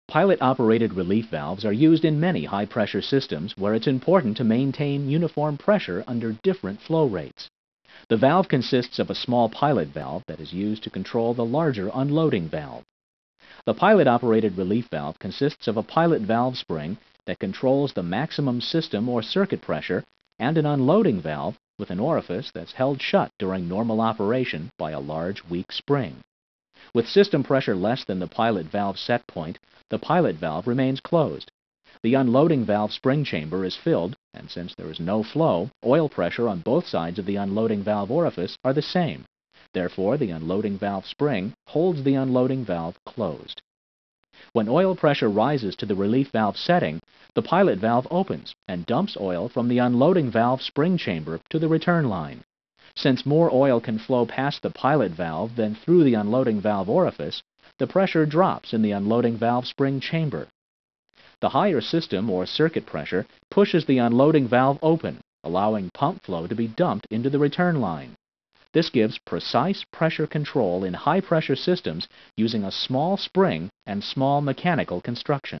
Pilot Relief Valve
ON this video explain about pilot relief valve .
Pilot operated valve are used in many high pressure systems ,where it’s important to maintain uniform pressure under different flow rate the valve consists of a small pilot valve that is used to control the large unloading valve